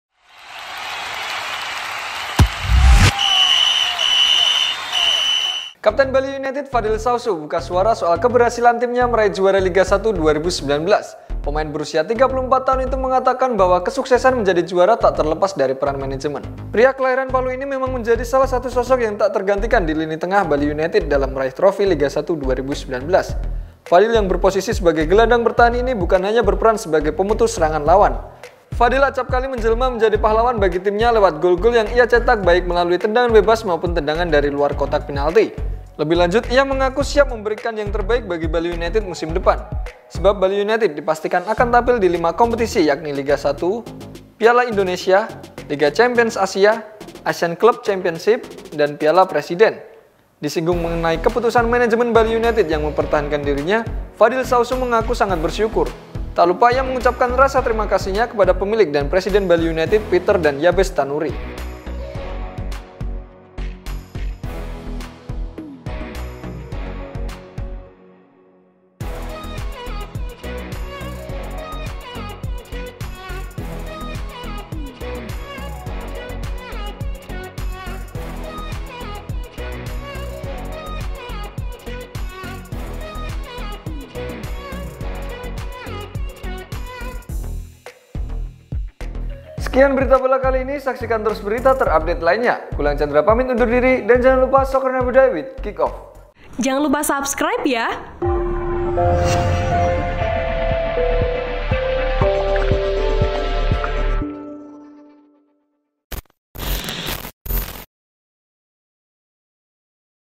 Sukses Juarai Liga 1 2019, Kapten Bali United Buka Suara
TRIBUN-VIDEO.COM - Kapten Bali United, Fadil Sausu, buka suara soal keberhasilan timnya meraih juara Liga 1 2019.

Pemain berusia 34 tahun itu mengatakan bahwa kesuksesan menjadi juara Liga 1 2019 ini tak terlepas dari peran manajemen tim asal Pulau Dewata ini.

"Bali United dari awal memperbaiki semuanya dari manajemen tata kelola tim tiap tahunnya, hingga menuju tim profesional," kata Fadil Sausu

"Hasilnya, tiap tahun semakin bagus dan hasilnya sekarang bisa juara," lanjutnya, dikutip SportFEAT.com dari Kompas.

Pria kelahiran Palu ini memang menjadi salah satu sosok yang tak terantikan di lini tengah Bali United dalam meraih trofi Liga 1 2019.

Fadil yang berposisi sebagai gelandang bertahan ini bukan hanya berperan sebagai pemutus serangan lawan.

Fadil acap kali menjelma menjadi pahlawan bagi timnya lewat gol-gol yang ia cetak baik melalui tendangan bebas maupun tendangan dari luar kotak penalti.

Lebih lanjut, pria bertinggi badan 1,75 meter ini mengaku siap memberikan yang terbaik bagi Bali United musim depan.

Sebab, Bali United dipastikan akan tampil di lima kompetisi, yakni Liga 1, Piala Indonesia, Liga Champions Asia atau Piala AFC, ASEAN Club Championship, dan Piala Presiden.

Disinggung mengenai keputusan manajemen Bali United yang mempertahankan dirinya, Fadil Sausu mengaku sangat bersyukur.

Tak lupa ia mengucapkan rasa terima kasihnya kepada pemilik dan presiden Bali United, Peter dan Yabes Tanuri.

"Yang pasti syukur alhamdulillah. Terima kasih kepada pak Pieter dan pak Yabes, serta pelatih yang masih mau pertahankan saya menjadi bagian keluarga Bali," ujar Fadil Sausu.

"Saya akan lebih bekerja keras lagi untuk Bali United," tutur Fadil mengakhiri.(*)

Artikel ini telah tayang di SportFEAT berjudul Skipper Bali United Bocorkan Kunci Kesuksesan Timnya Juarai Liga 1 2019